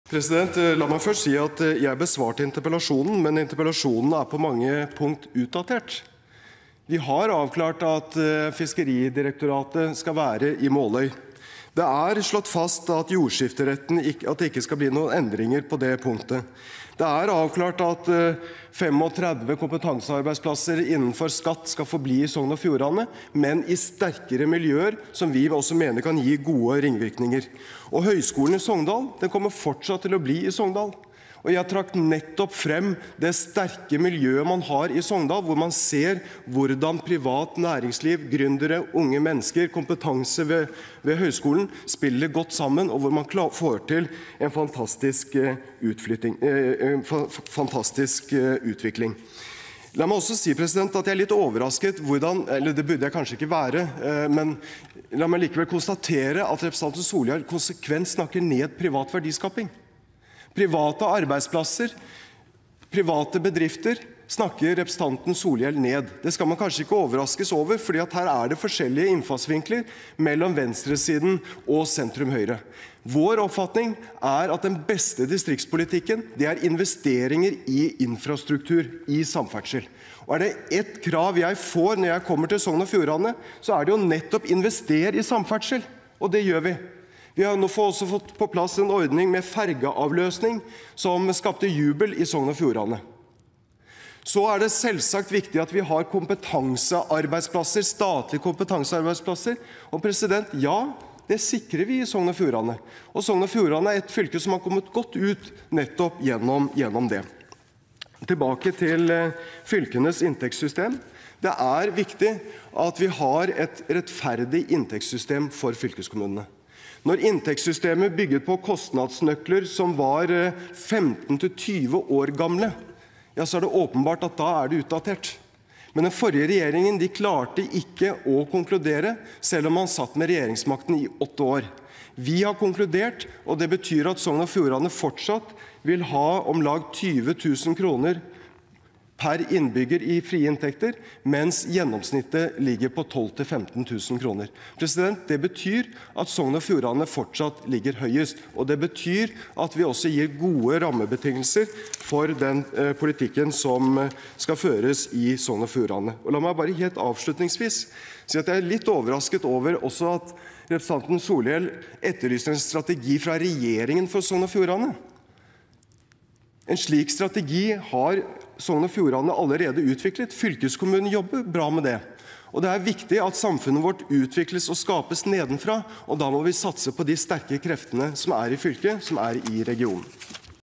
21.05.2015 Sak 7 2 Jan Tore Sanner
Om å utvikle og styrke Sogn og Fjordane fylke, med bakgrunn i at regjeringa har lansert ei rekkje reformer som kan bety omfattande sentralisering bort frå fylket